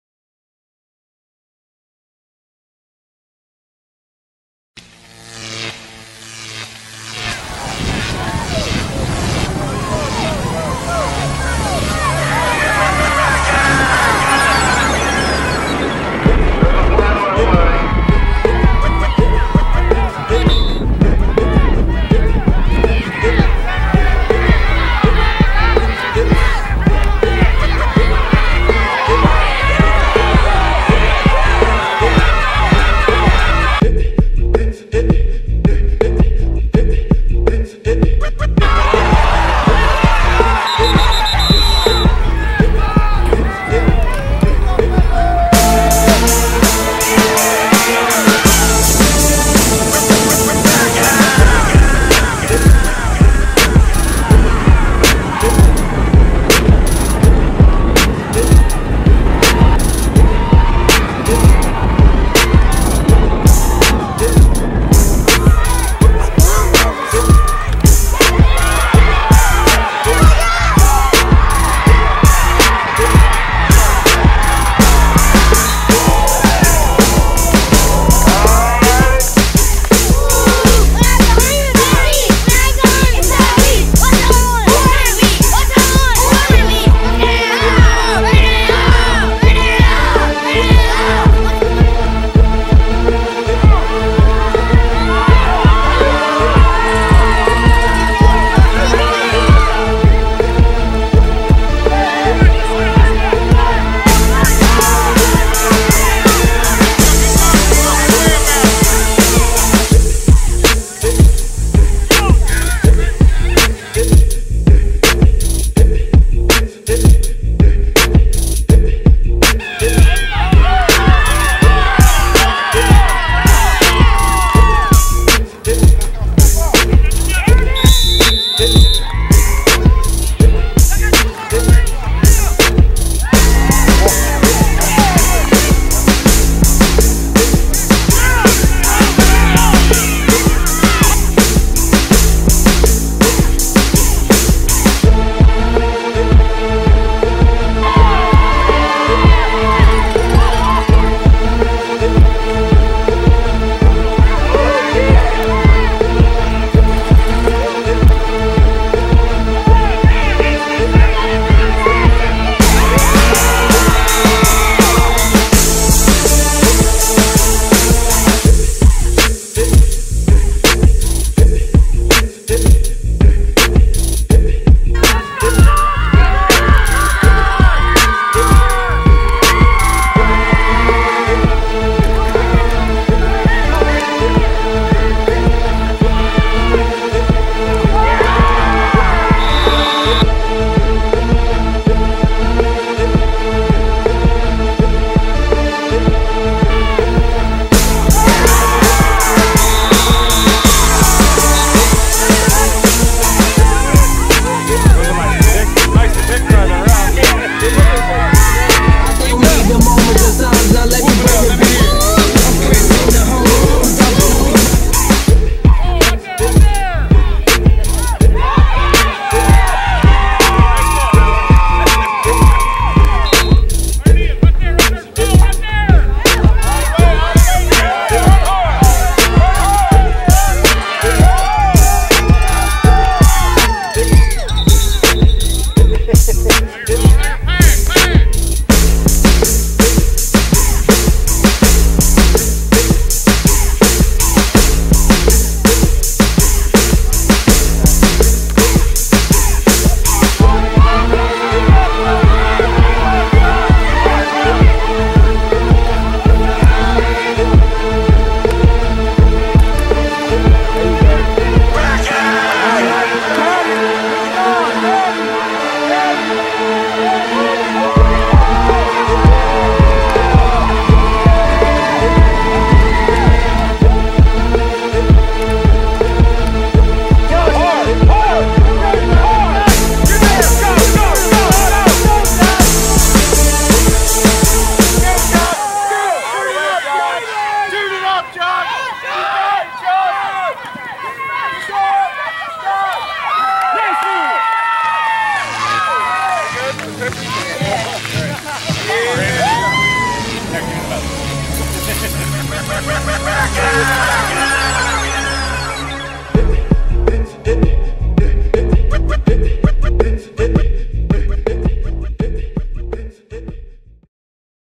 7 year old Football Player Highlight Josh Evans Ohio Football
Josh Evans QB of the Mid West National Champions West Chester Outlaws football, 7U - 7 & UNDER youth football in Ohio.  Team is rank as one of the top 7 & under teams in the country JC has a record of 34-0 as the starting QB of the outlaws.

3 SuperBowl Championships
2012 RockyTop Champions